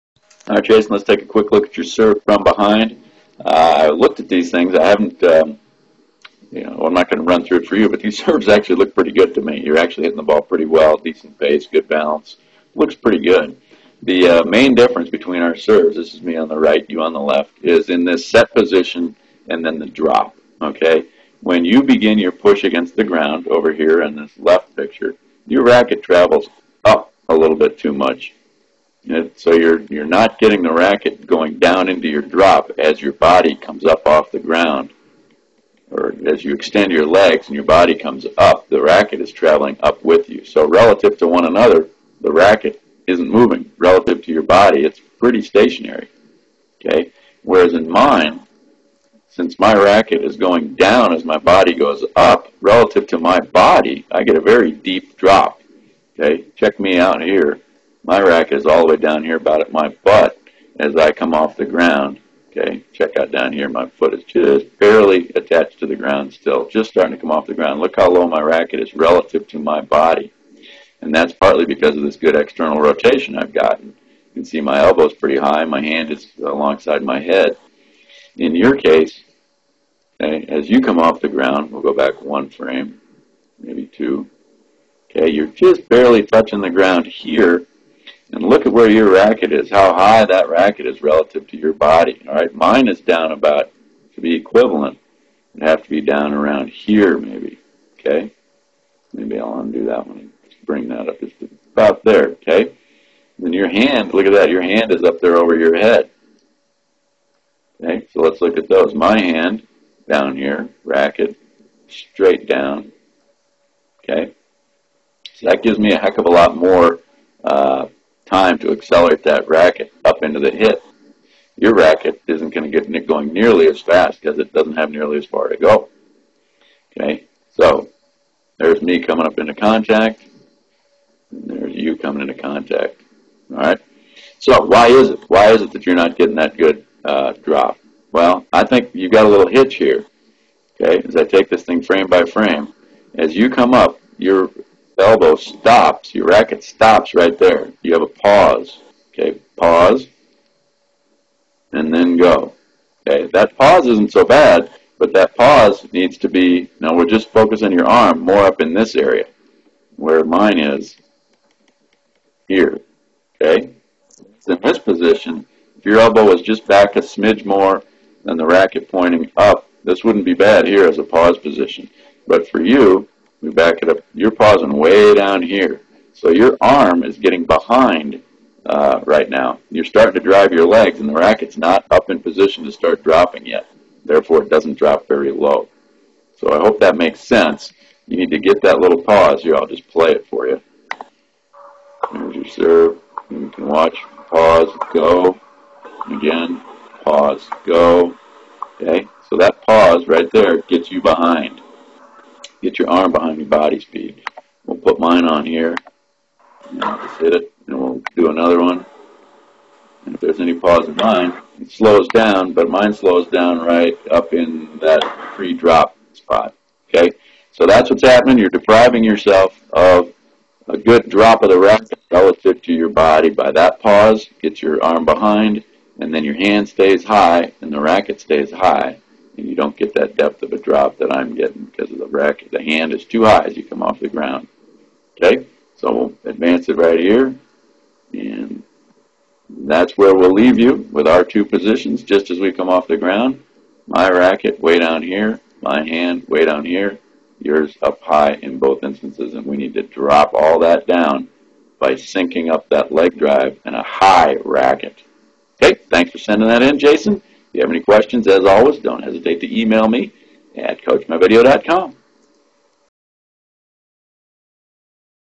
Tennis Serve - Personalized Lesson from Bob
Coach Bob identifies the next area for Jason to focus on his serve